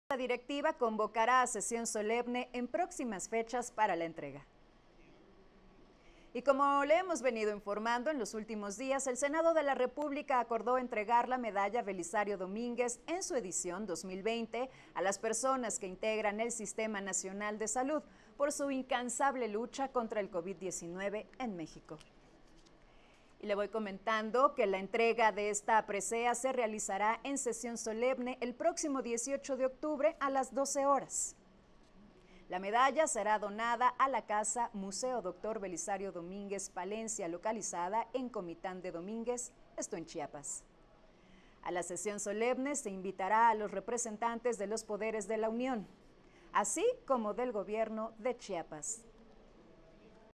Entregarán Medalla Belisario Domínguez 2020
El Senado de la República acordó entregar la Medalla Belisario Domínguez en su edición 2020 a las personas que integran el sistema nacional de salud, por su incansable lucha contra el Covid-19 en México, en sesión solemne, el próximo 18 de octubre, a las 12 horas.